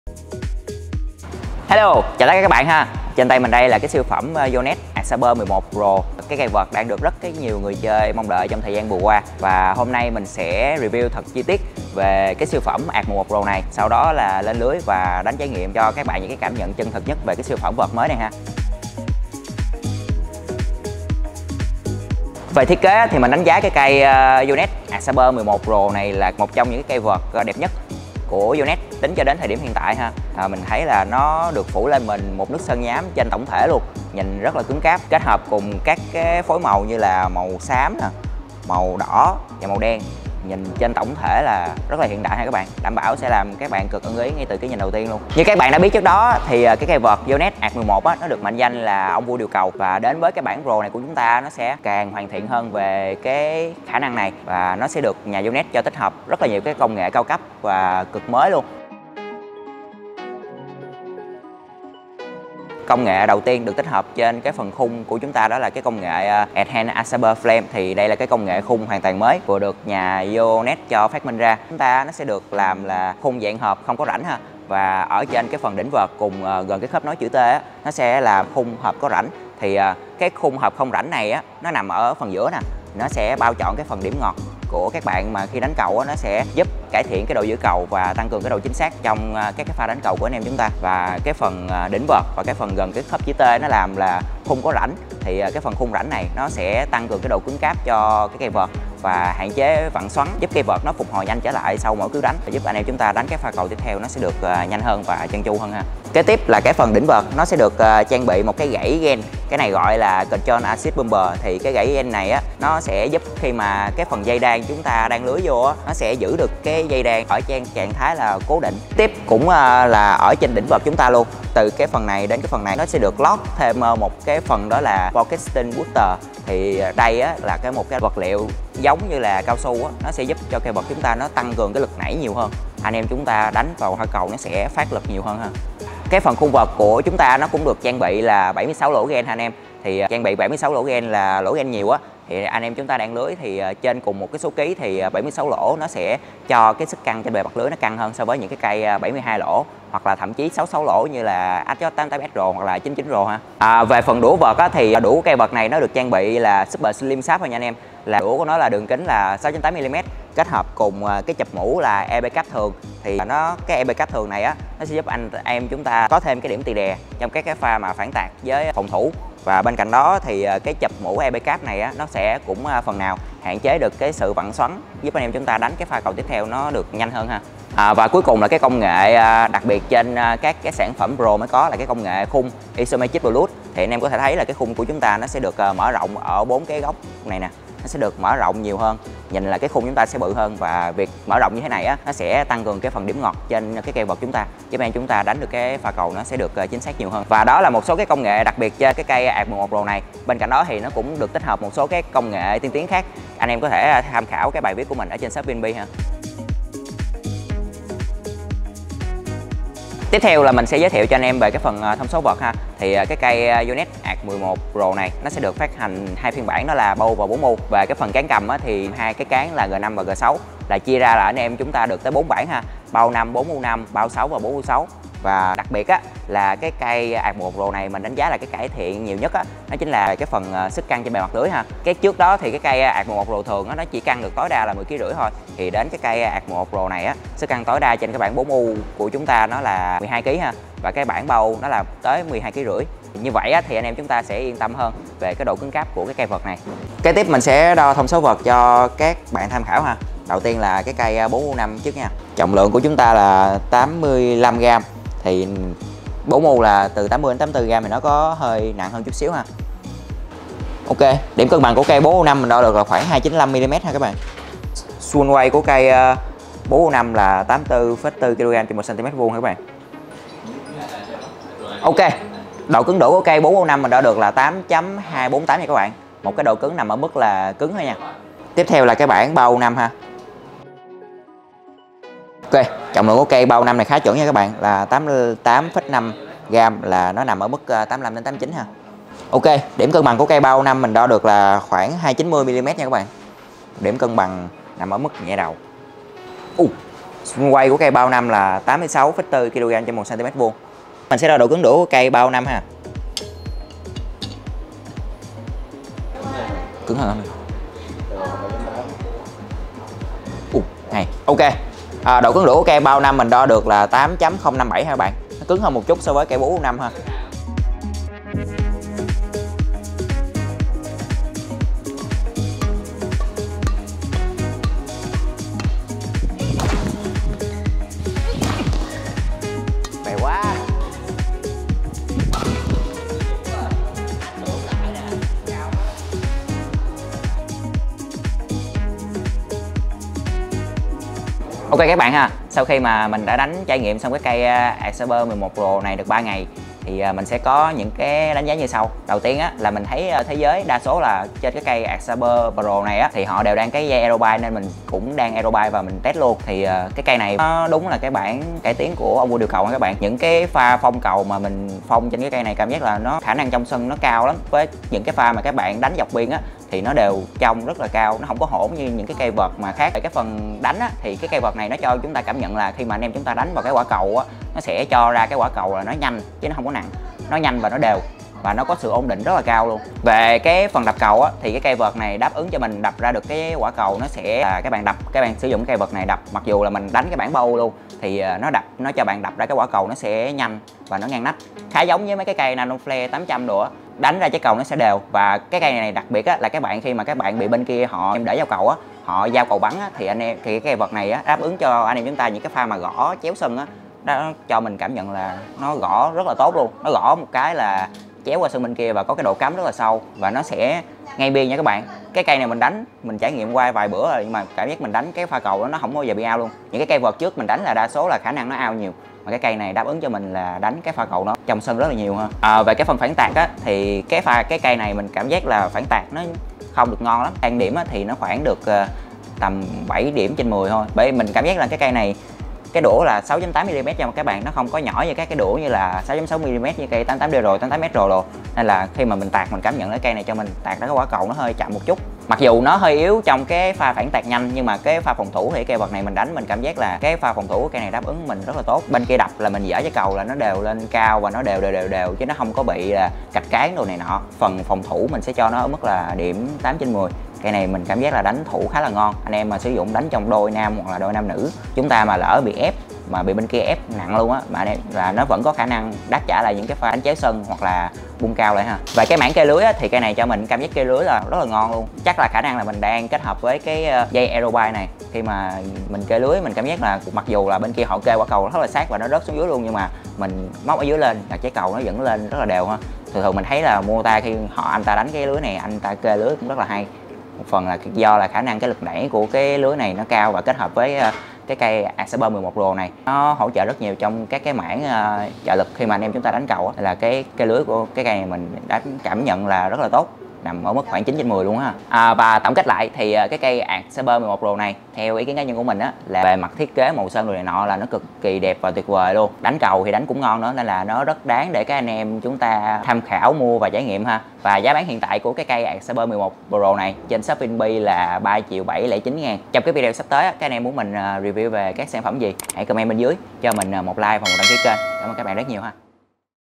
Liệu Ông Vua Điều Cầu Đã Được Nâng Cấp??? | Review Vợt Cầu Lông Yonex Arcsaber 11 Pro
Vợt cầu lông Yonex Arcsaber 11 Pro 2022 đã được phát hành với 2 phiên bản 3U và 4U được sơn xám với phối màu Ngọc Trai Xám cho tổng thể cái nhìn hiện đại và cứng cáp.
Với nhiều cải tiến công nghệ mới: ENHANCED ARCSABER FRAME - CONTROL-ASSIST BUMPER - POCKETING BOOSTER - ISOMETRIC PLUS giúp cải thiện rõ rệt về độ chính xác cùng khả năng đập cầu so với người đàn em tiền nhiệm Arcsaber 11 và hứa hẹn sẽ cho các lông thủ những trải nghiệm tốt nhất trên sân đấu.

Và trong video này, bạn Sĩ Phương sẽ review và đánh giá một cách đầy đủ về siêu phẩm Yonex Arcsaber 11 Pro này nhé!!!

Xem thêm bài viết:

- Yonex Arcsaber | Dòng vợt cầu lông Yonex thiên về điều cầu

- YONEX ARCSABER 11 PRO: Bản Cải Tiến Vượt Bậc Của Ông Vua Điều Cầu

Nếu mọi người quan tâm về những nội dung khác về bộ môn CẦU LÔNG và muốn mình làm Video thì hãy COMMENT phía dưới nhé !